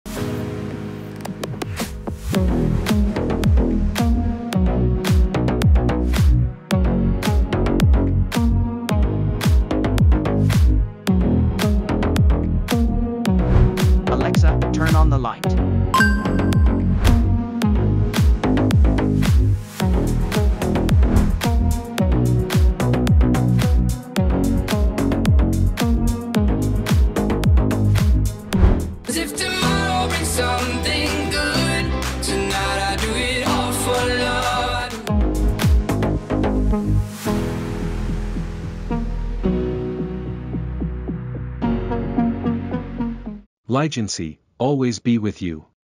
What features does the Ligency 4 Inch/9W Smart Recessed Lighting have?
【RA≥90, 120LM/W】Ligency LED smart recessed lighting uses the latest technology to achieve high luminous flux (high luminous index, high lumens) with a high apparent index of 90 and 120 lumens/W. High color rendering index, uniform light color, high efficiency of light output, longer use. The housing of smart WiFi recessed lights adopts an anti-light leakage structure to ensure maximum light output.

【Easier Installation】The J-Box of Ligency ultrathin canless ceiling light is designed with three wiring ports, allowing for multi-angle installation to meet different needs. It makes the installation process easier, safer and faster. In addition, the spring installation of LED smart downlight uses thickened materials that can solve the common problem of spring breakage at the installation.

【Three Types of Control】Ligency WiFi LED Retrofit Downlight has three control methods: ① Alexa and Google Assistant ② Smart Life APP ③Remote Controller. Through a simple conversation or APP control, you can control the switch of smart wafer downlight, change the color or adjust the brightness. Our smart lighting also comes with a remote control, so if your phone is not with you, you can use the remote controller to do the same. (Note: The remote control does not come with a battery)

【16 Million Colors】Smart Canless Wafer Downlight supports warm white and cool white, from 2700K to 6500K, providing 16 million colors with 1%-100% adjustable brightness. With the Smart Life App, you can express your mood by giving the LED downlight DIY different colors to suit various scenarios, such as a party between friends or a romantic date between lovers!

【Music Synchronization】Ligency Canless Ceiling Light will change color with the rhythm of the phone music or outside sounds to create a different atmosphere. Note: This function only applies to the APP, the product does not have a built-in microphone.

【More functions to use in Smart Life APP】Download Smart Life's APP to unlock more functions of Ligency smart downlight. Exclusive schedule, group control lights, memory function, DIY lighting scenes, and share devices .